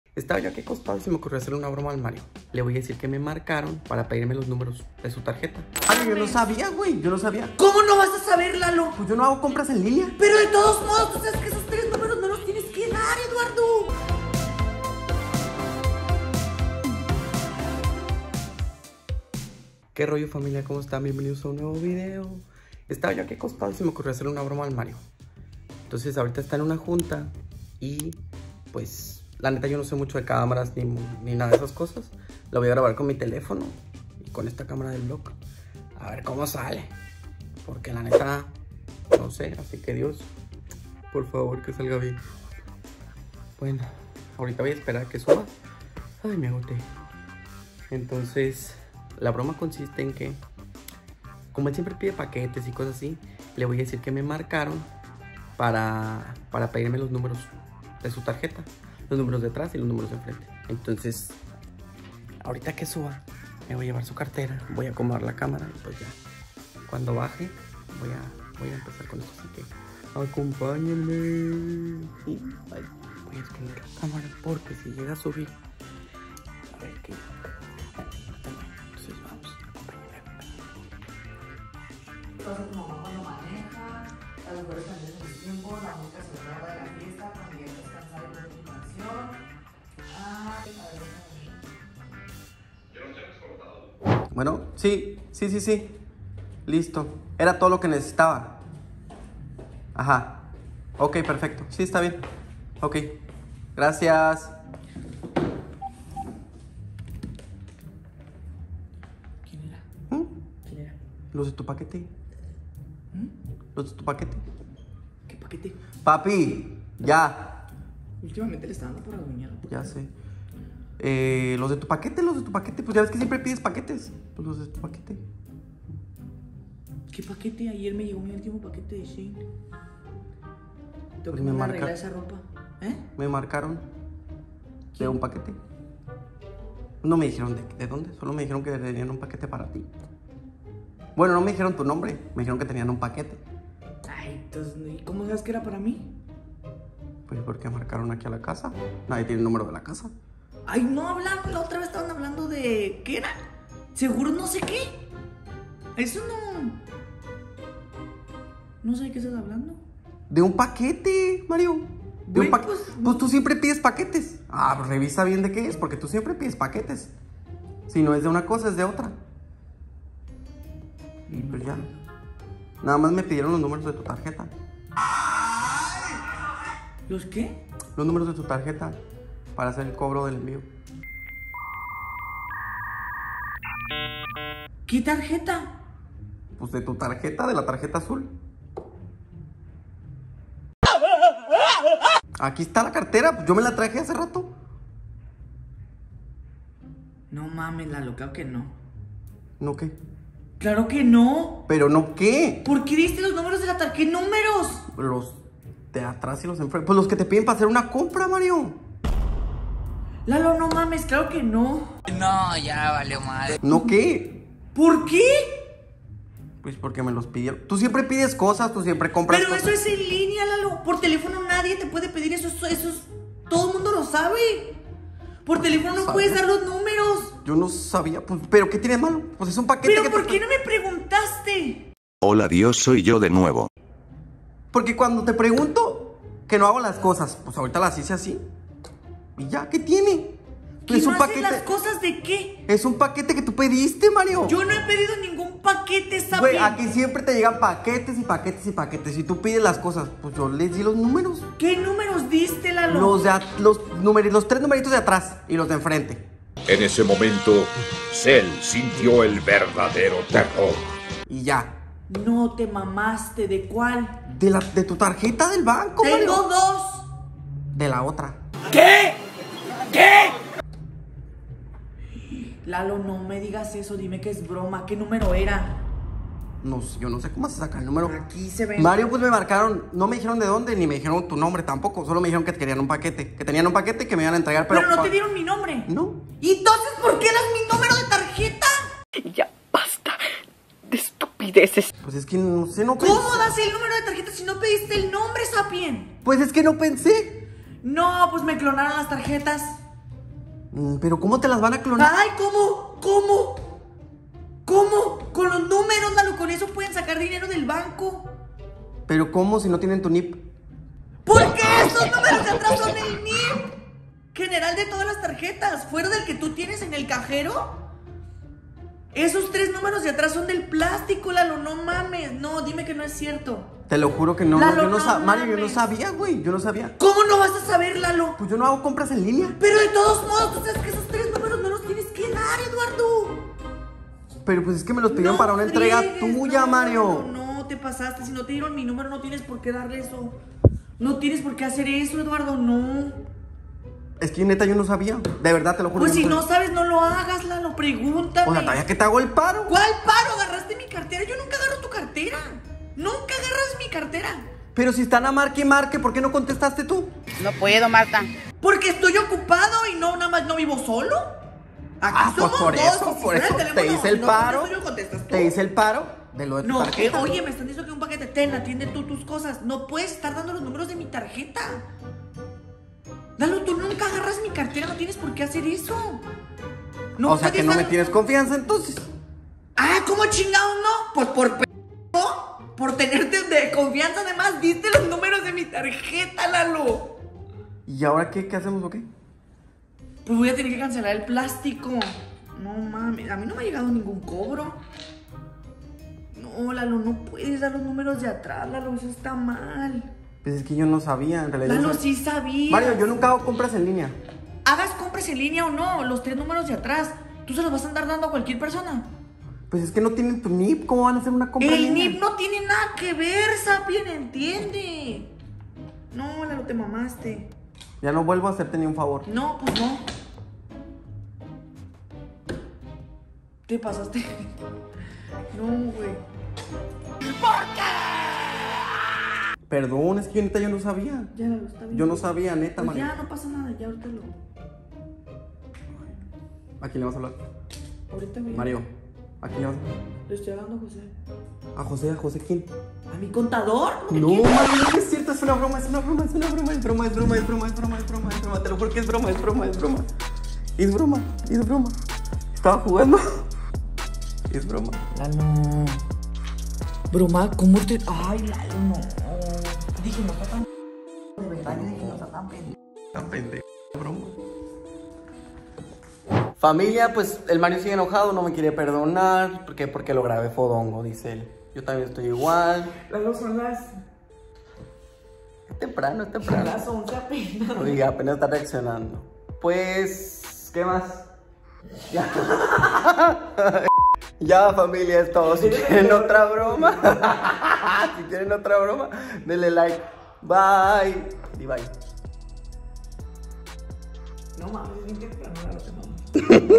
Estafaron a mi NOVIO y nos clonaron las tarjetas. BROMA 😱 | Mario Aguilar
Todo parecía ser un día tranquilo cuando de repente me doy cuenta que estafaron a mi novio por teléfono y nos clonaron todas nuestras tarjetas bancarias, al final descubrí que se trataba de una broma ¡Gracias al cielo! y cuando lo enfrenté pasó esto...